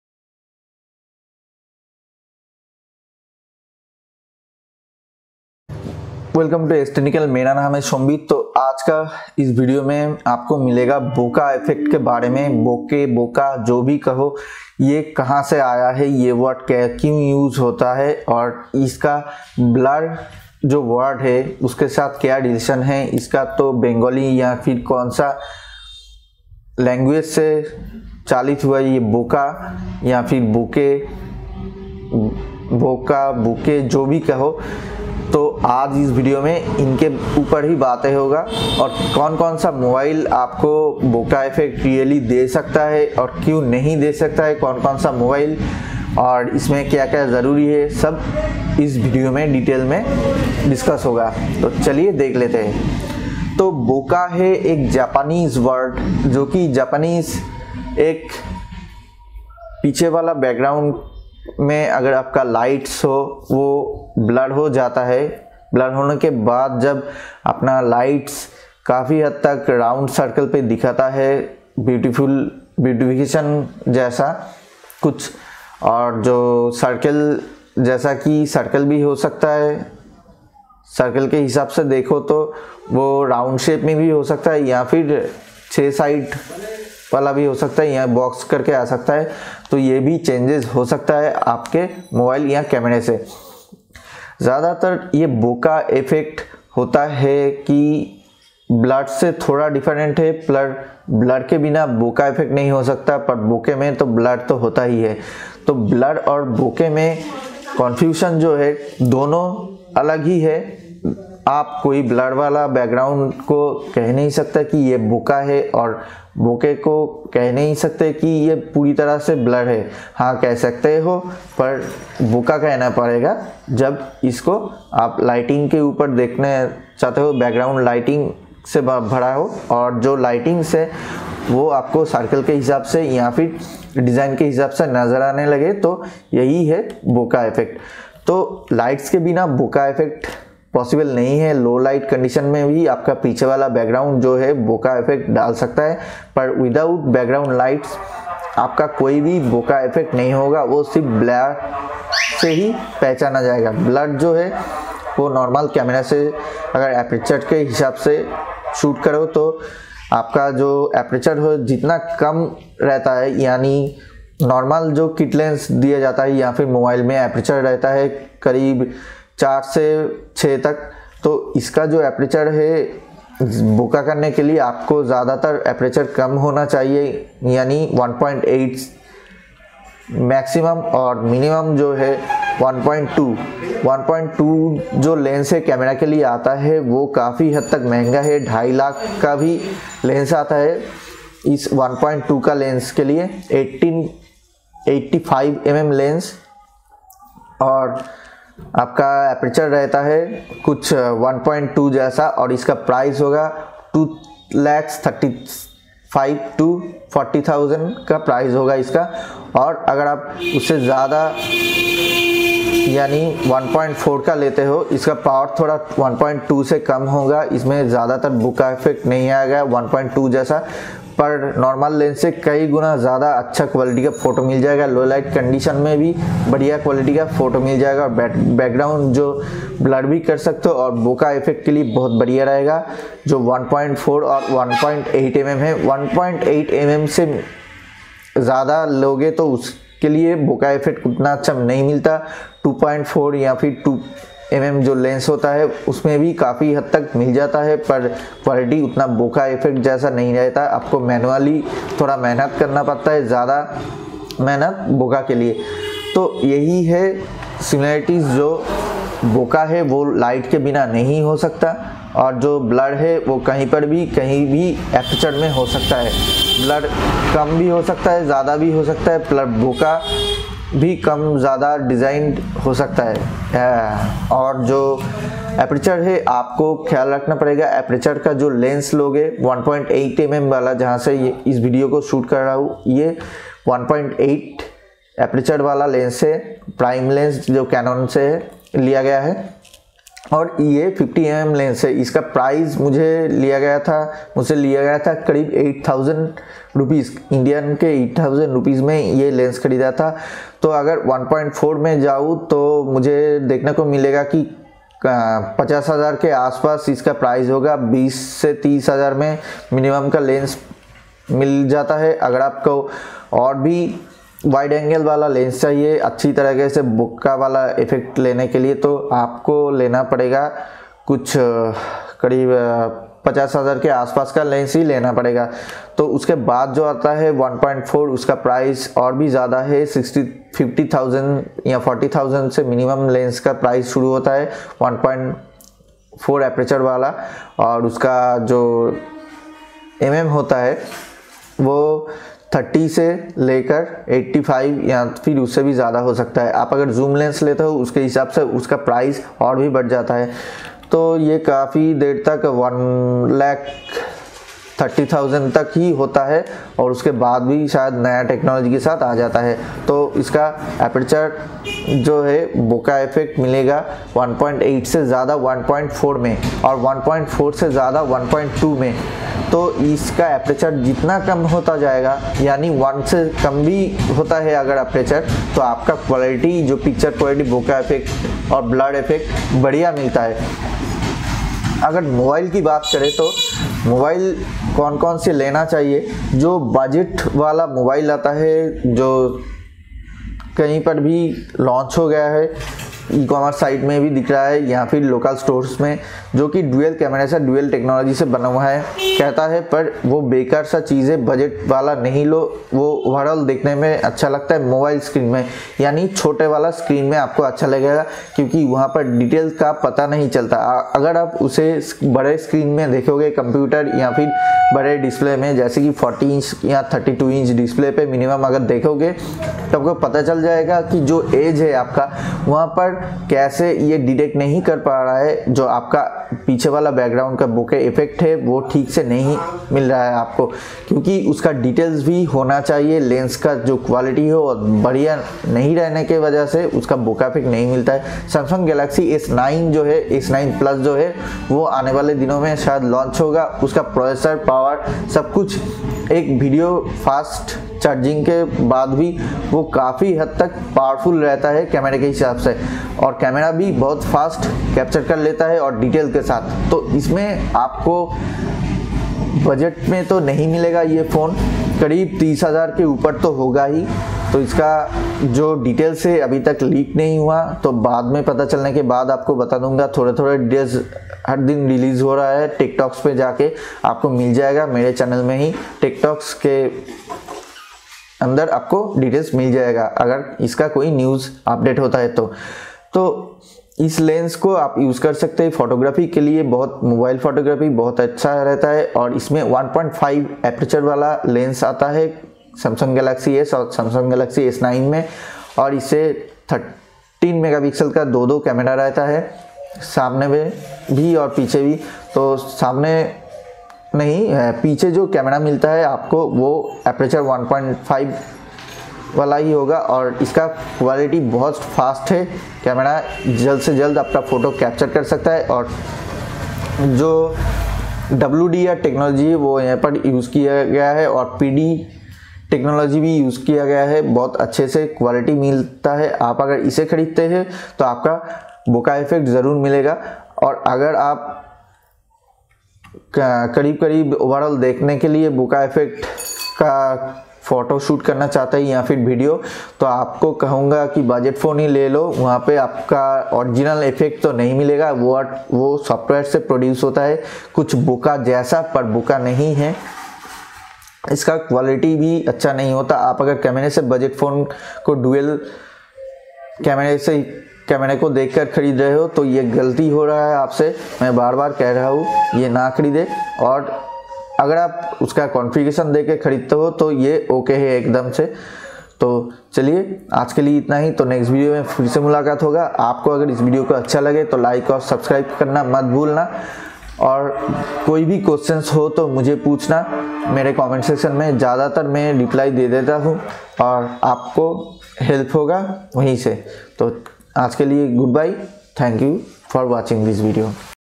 Bokeh | What Is Bokeh? | What Is Blur? | Required Lens | Aperture For Bokeh Effect
Dosto aap logo ko is video me bokeh effect jo ki Japanese word hai.Is ka real meaning keya hai or blur se is ta keya nata hai is video ko puri tarikese dekhne bad pata chal jayega.Accha lage to like krna or is tarikeka aur vi videos dekhna chate ho to subscribe kro.

S Technical
Sambit Halder